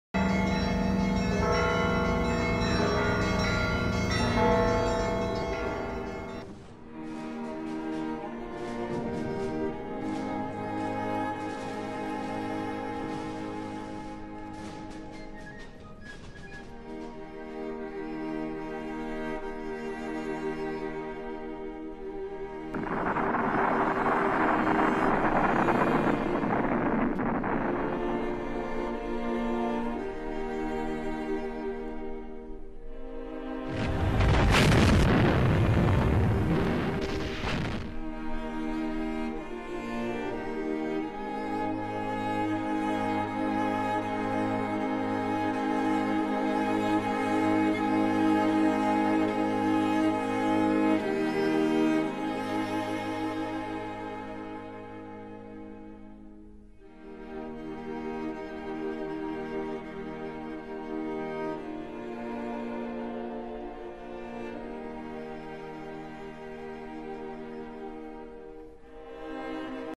Christ the Savior. History of the twice built Moscow´s catheral
In 1931, the tallest temple of Orthodox Christianity was dynamited. The center of power of the patriarch of Moscow was demolished to build a great Soviet monument instead. More